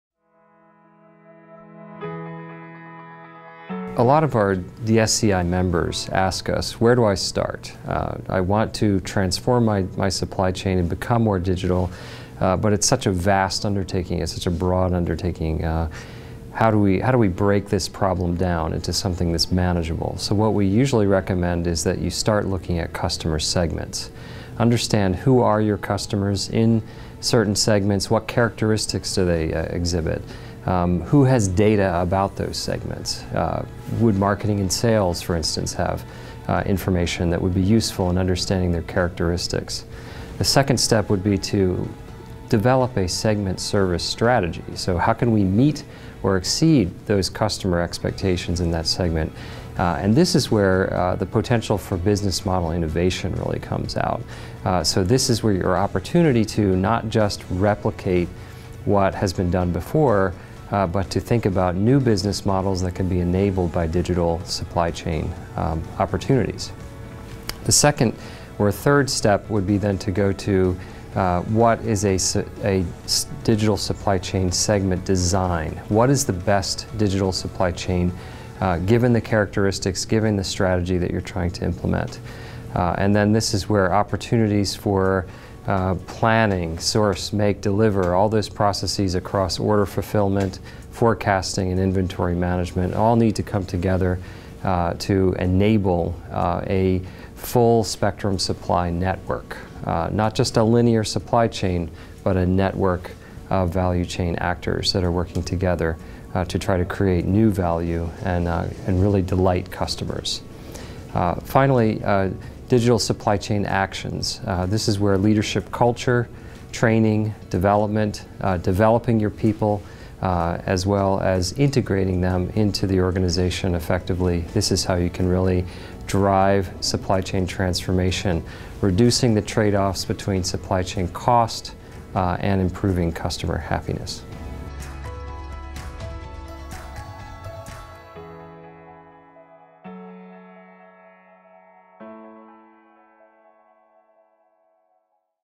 Steps To Jump Start The Supply Chain Transformation Journey
Dave Kurz, Research Fellow at Digital Supply Chain Institute shares necessary steps to start the supply chain transformation at the 8th Executive Leadership Forum at SMU Cox Business School in Dallas, Texas. Around 70 supply chain executives from various different industries attended the DSCI's eighth forum in October 2019.